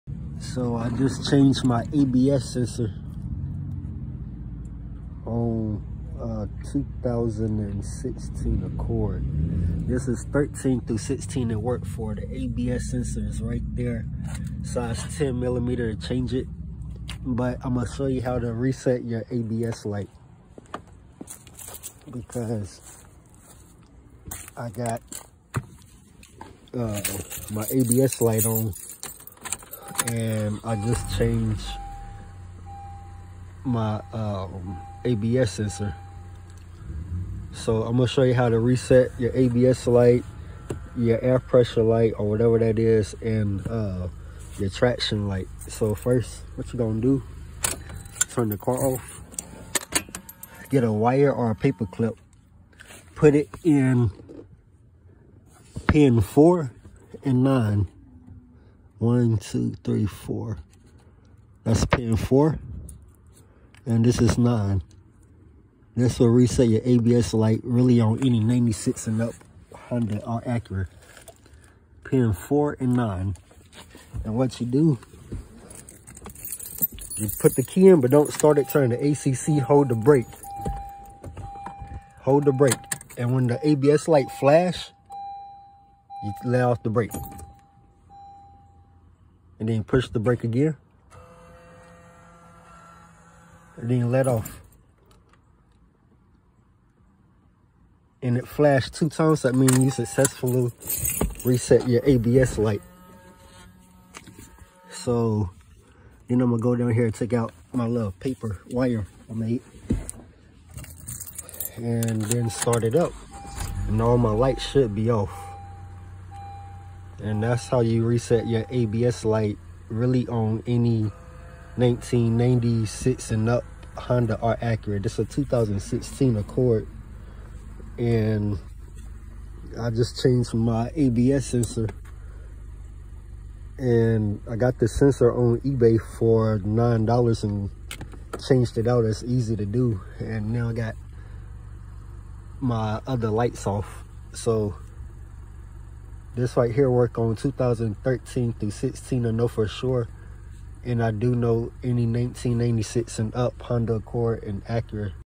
Reset ABS light 2013 - 2016 Honda acurs
ABS sensor size 10 mm socket and how to change ABS sensor and reset light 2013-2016 accord￼

like & sub